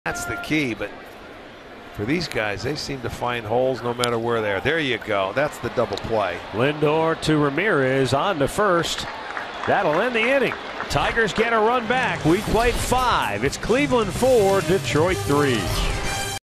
CLE@DET: Otero gets out of the 5th with a double play
Dan Otero gets Victor Martinez to ground into a double play to end the 5th inning, preserving the Indians' one-run lead